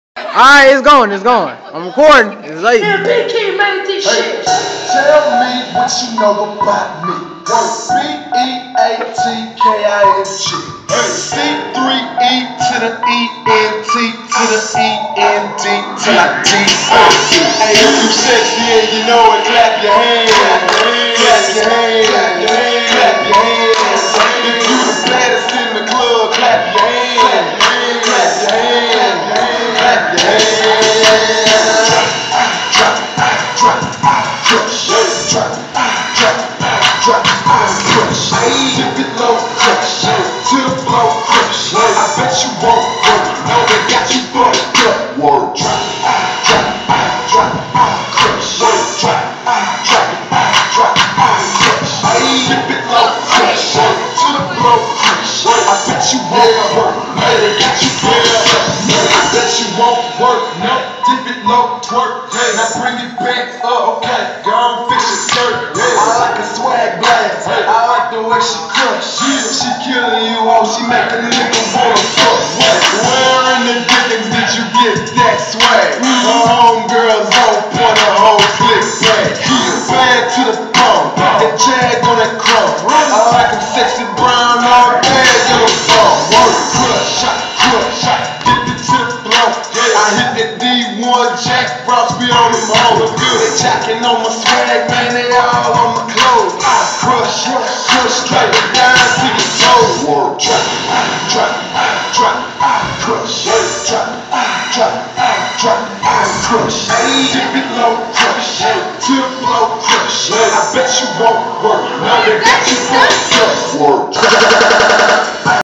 601 Chicks (Jackson, MS) dance rehearsal
Jackson, Mississippi's own 601 Chicks rehearsing a dance at Regent Teen Center